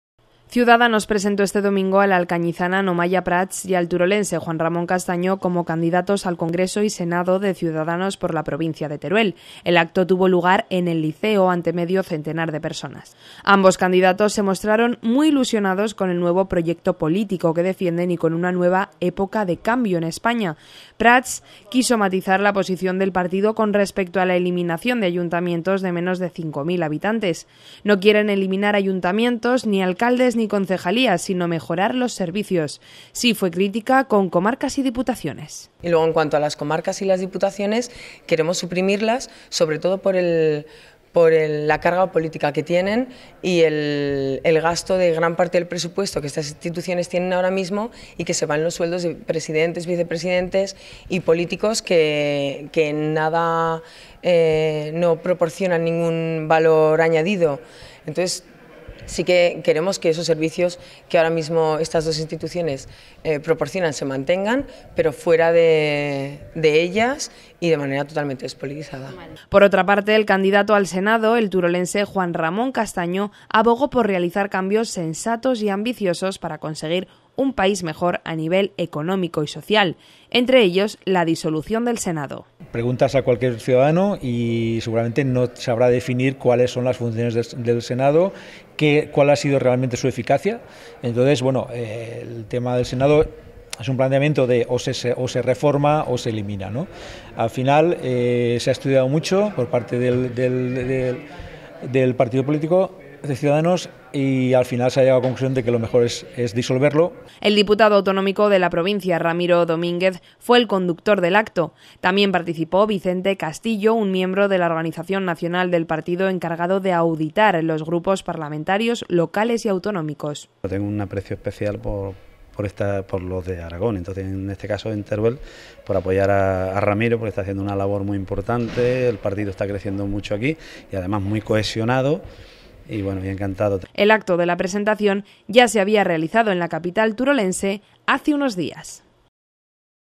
La Comarca.tv - Presentación candidatos CIUDADANOS por Teruel
CIUDADANOS  presentó en Alcañiz al turolense Juan Ramón Castaño y a la alcañizana Nomaya Prats como candidatos al al Senado y Congreso por la provincia de Teruel. El acto tuvo lugar en el liceo de la ciudad ante medio centenar de personas.